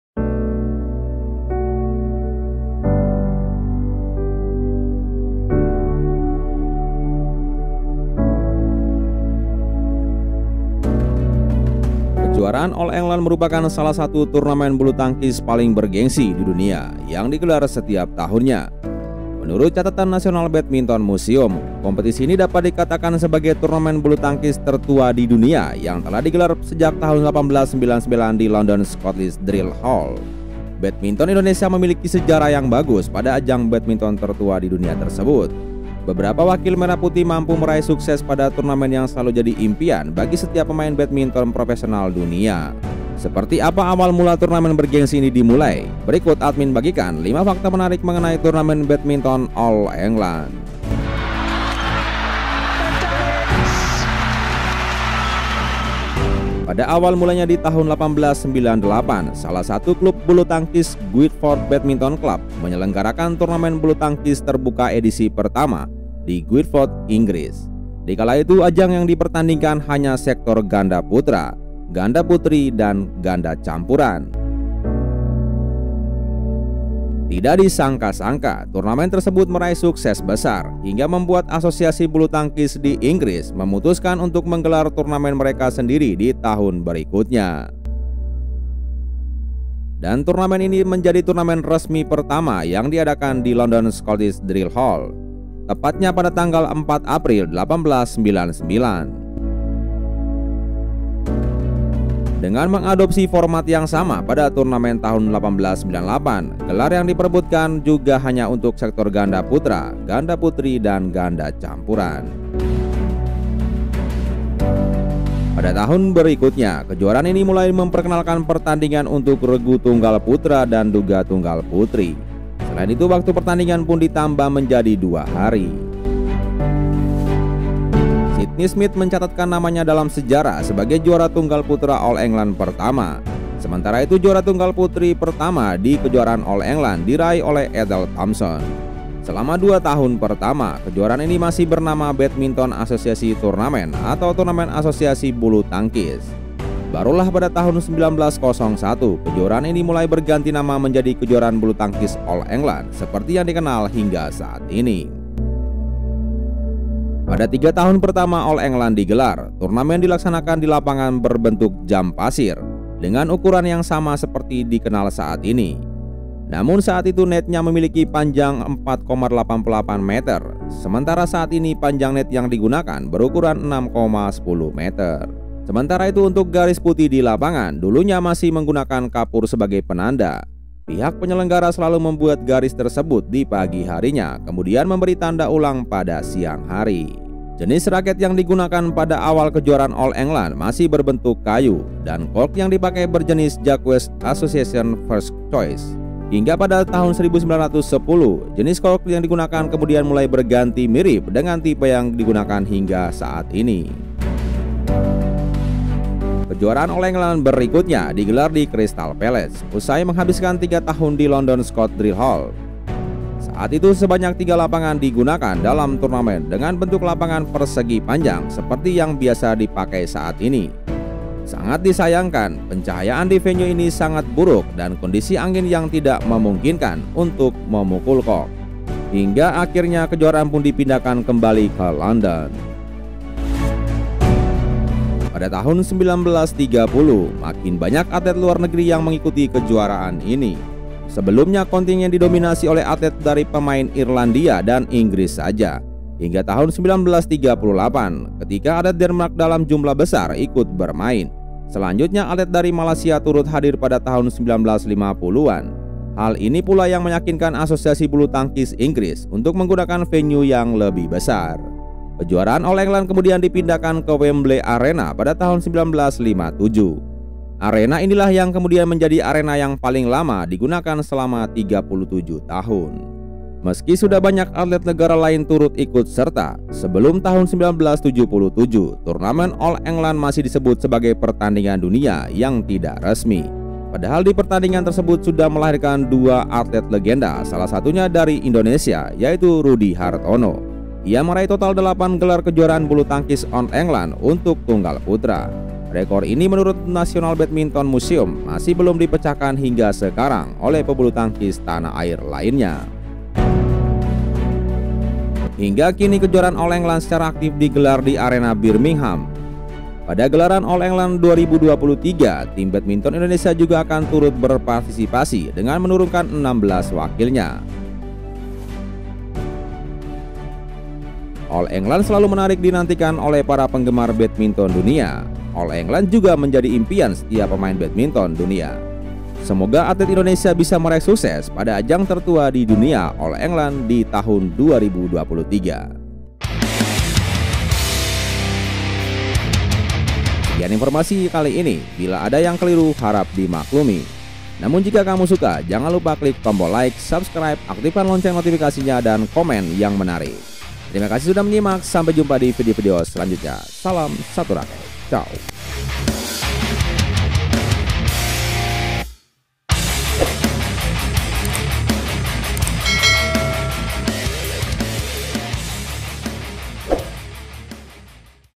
Lima fakta menarik turnamen Badminton All England
Kejuaraan All England merupakan salah satu turnamen bulu tangkis paling bergengsi di dunia yang digelar setiap tahunnya. Menurut catatan National Badminton Museum, kompetisi ini dapat dikatakan sebagai turnamen bulu tangkis tertua di dunia yang telah digelar sejak 1899 di London Scottish Drill Hall. Badminton Indonesia memiliki sejarah yang bagus pada ajang badminton tertua di dunia tersebut. Beberpaa wakil merah putih mampu meraih sukses pada turnamen yang selalu jadi impian setiap pemain badminton professional dunia. Seperti apa awal mula turnamen bergengsi ini dimulai ? Berikut admin bagikan 5 fakta menarik mengenai turnamen Badmiotn All England.